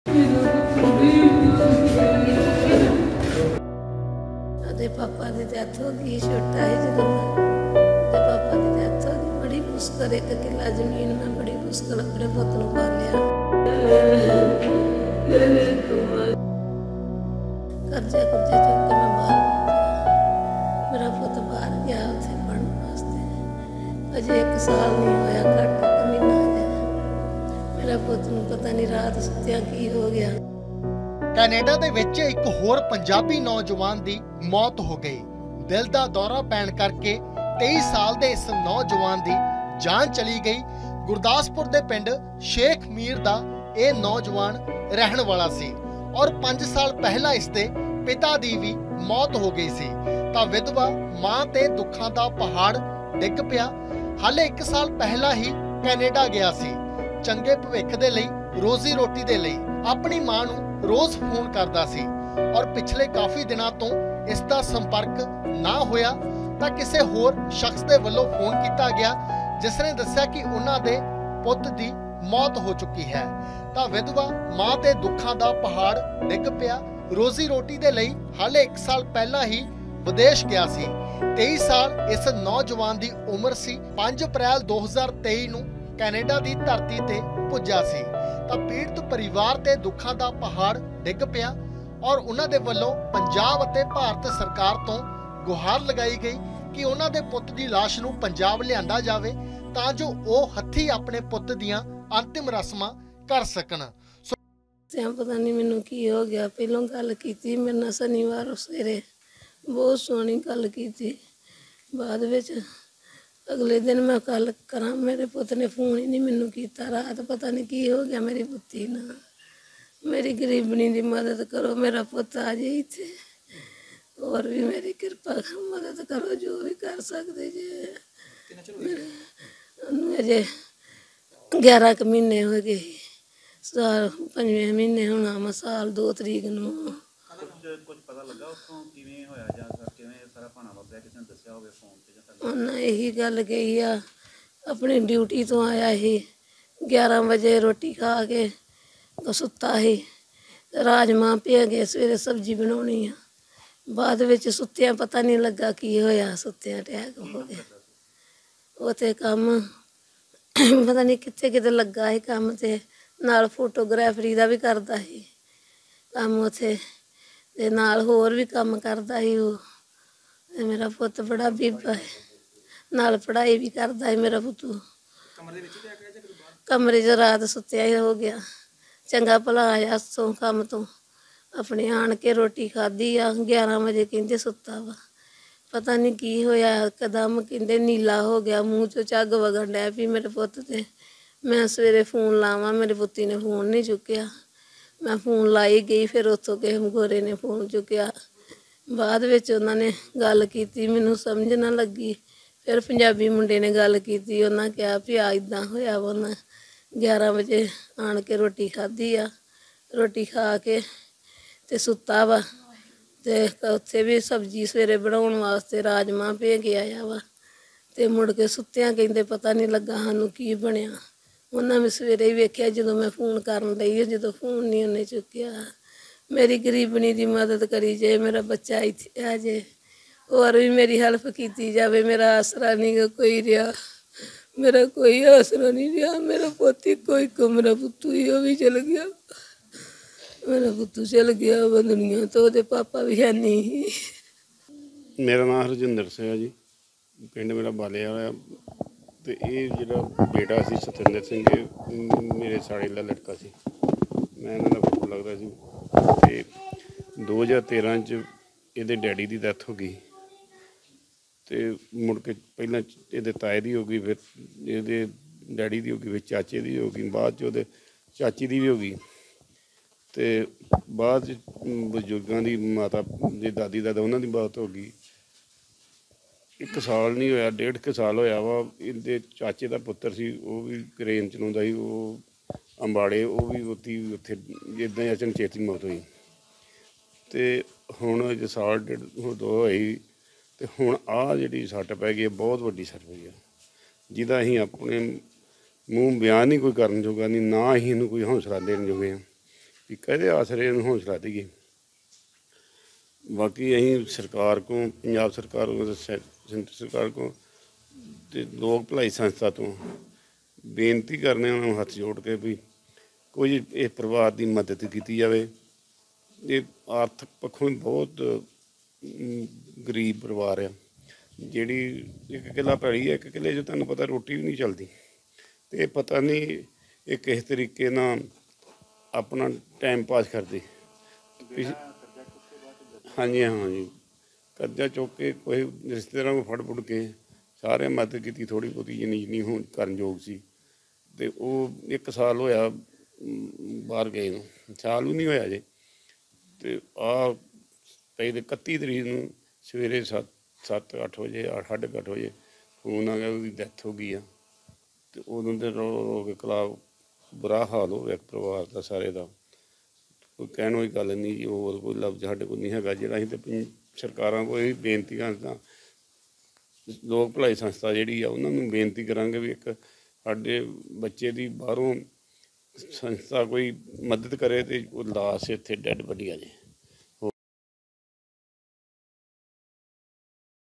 Gurdaspur News | ਸੁੱਤਾ ਰਹਿ ਗਿਆ ਵਿਧਵਾ ਮਾਂ ਦਾ ਇੱਕੋ-ਇੱਕ ਸਹਾਰਾ, ਕੈਨੇਡਾ 'ਚ ਨਿੱਕਲ ਗਈ ਜਾਨ
ਗੁਰਦਾਸਪੁਰ ਦੇ ਥਾਣਾ ਕਲਾਨੌਰ ਅਧੀਨ ਪੈਂਦੇ ਪਿੰਡ ਸ਼ੇਖ ਮੀਰ ਦੇ ਰਹਿਣ ਵਾਲੇ ਨੌਜਵਾਨ ਦੀ ਕੈਨੇਡਾ 'ਚ ਦਿਲ ਦਾ ਦੌਰਾ ਪੈਣ ਕਾਰਨ ਮੌਤ ਹੋ ਗਈ। ਇਸ ਕਾਰਨ ਵਿਧਵਾ ਮਾਂ ’ਤੇ ਦੁੱਖ ਦਾ ਪਹਾੜ ਡਿੱਗ ਪਿਆ ਹੈ।